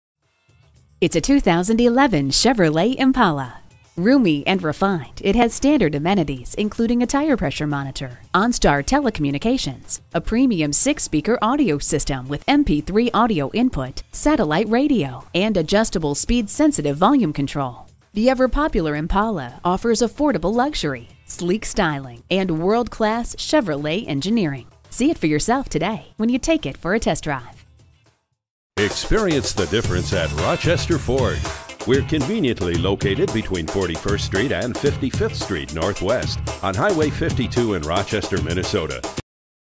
Used 2011 Chevrolet Impala Rochester MN Winona, MN PA9006 - SOLD
Year: 2011
Make: Chevrolet
Model: Impala
Trim: LT
Engine: 3.5L V6 SFI Flex Fuel
Transmission: 4-Speed Automatic with Overdrive
Color: Silver
Interior: Ebony
Mileage: 106613
Stock #: PA9006
VIN: 2G1WB5EK8B1145184
2011 Chevrolet Impala LT Silver New Price! Local Trade, Leather, Cruise Control, ABS brakes, Alloy wheels, Power windows, Preferred Equipment Group 2FL, Remote keyless entry, Steering wheel mounted audio controls, Traction control. We look forward to earning your business!

Address:
4900 Highway 52 North